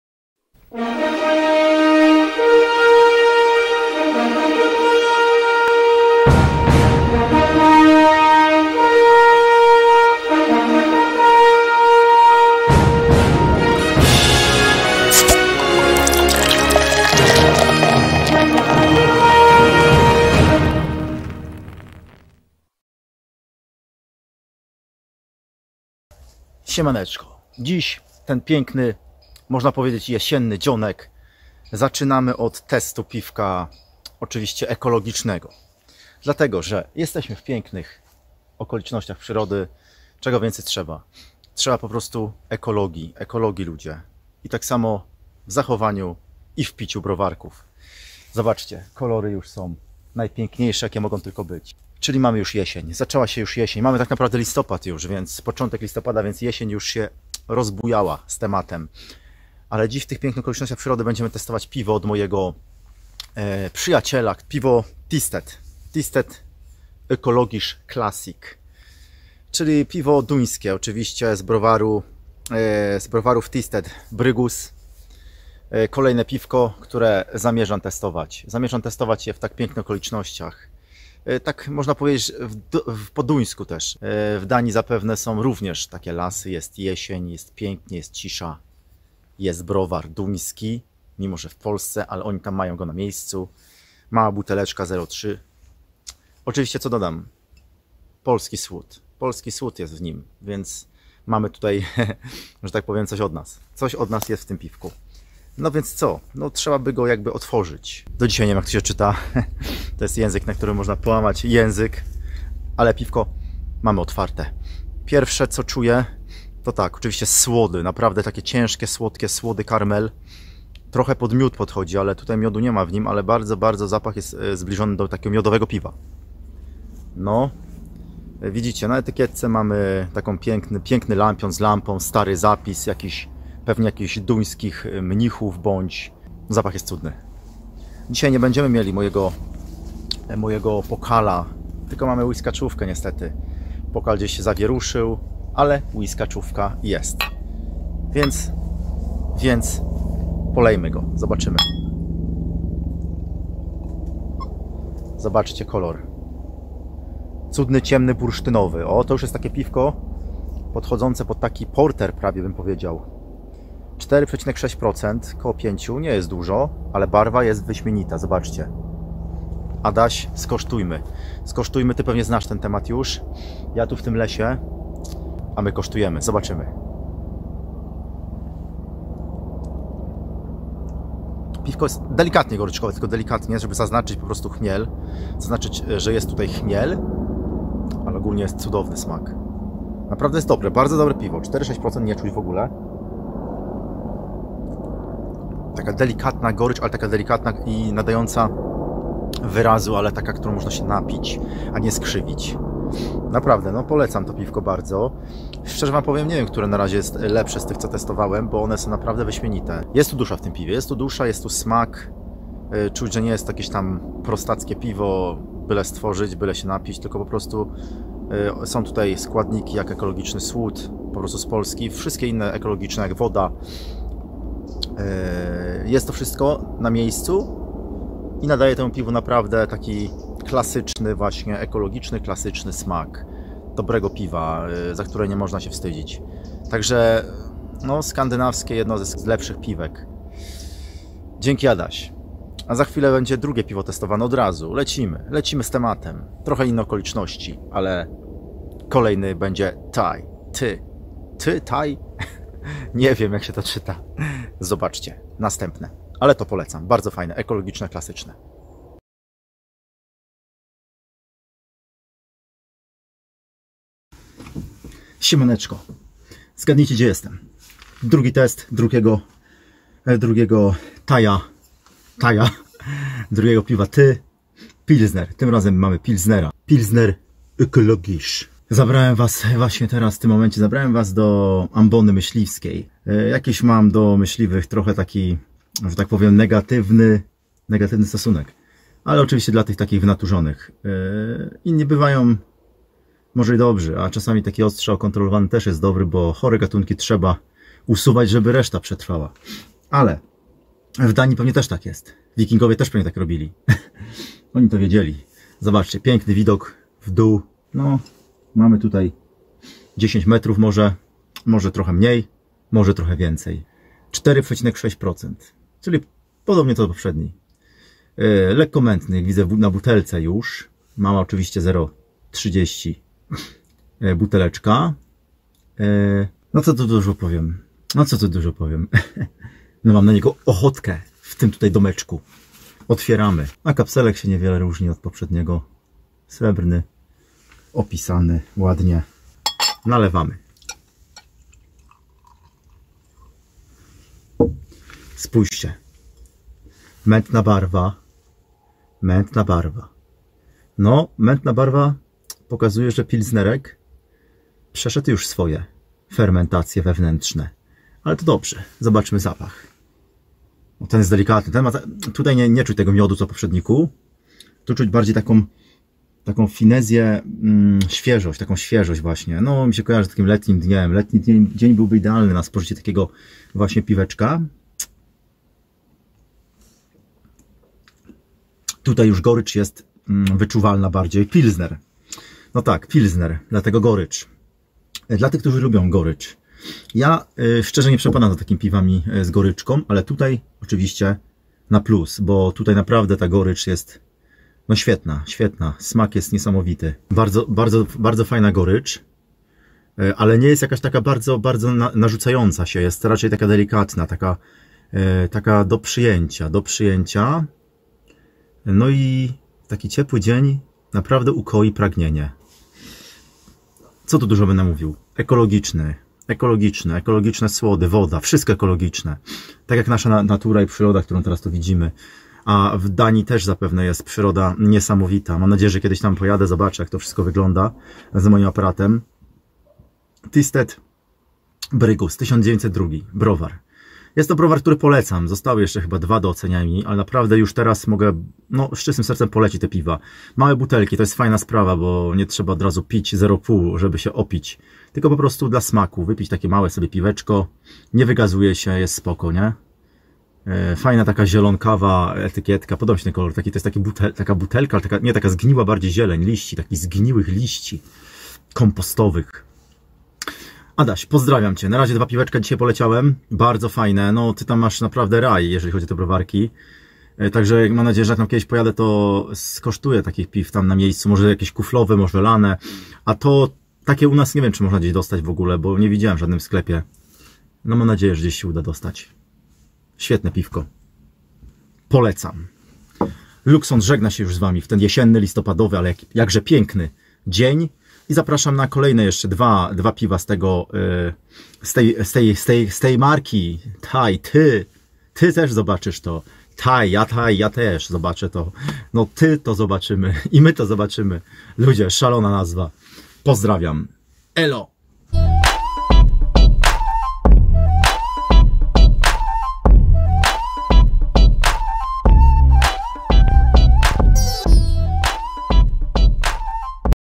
🍺Thy Pilsner Økologisk & Økologisk Classic🍺
Jedno i drugie piwko smakują wybornie, choć pierwsze jest dla tych, którzy doceniają słodkie nuty natomiast drugie jest typowo goryczkowe:) Dwa piwka ... dwa różne światy... duńskie światy:P